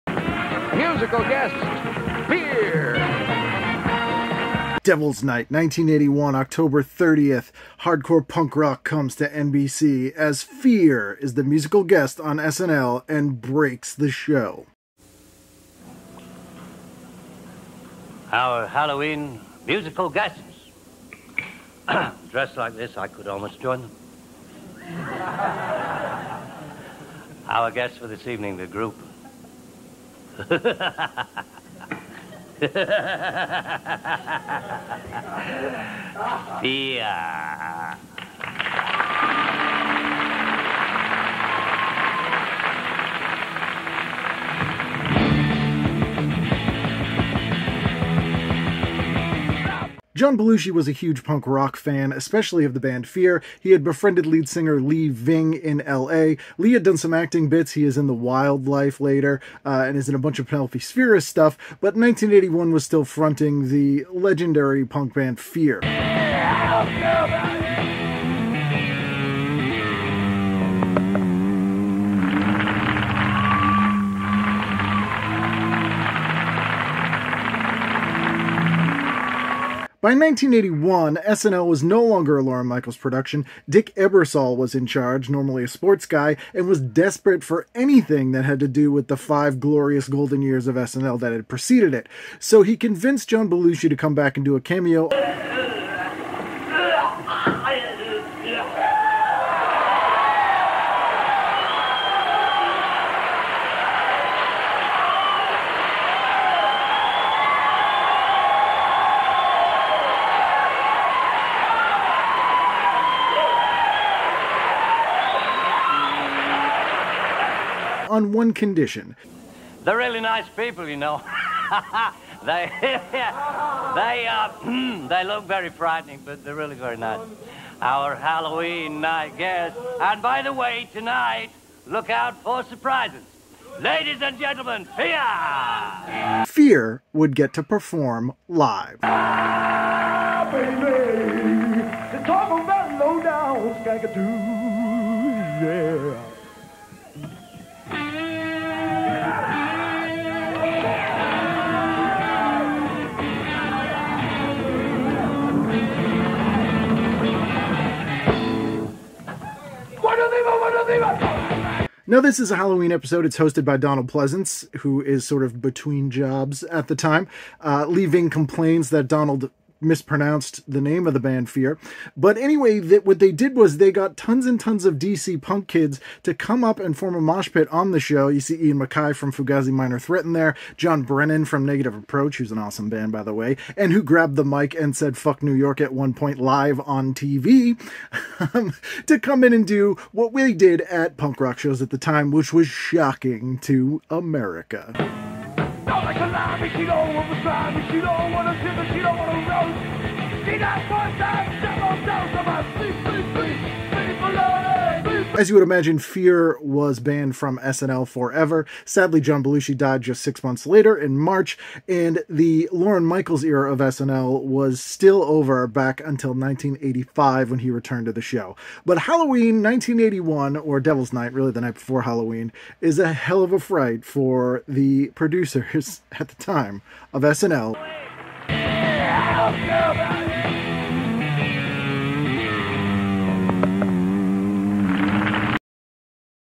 TV Guidance Counselor : When Fear gripped SNL
October 30, 1981 John Belushi does a silent cameo on the post Lorne Michaels SNL in exchange for a guest spot for his friends, the punk band Fear. Chaos ensued.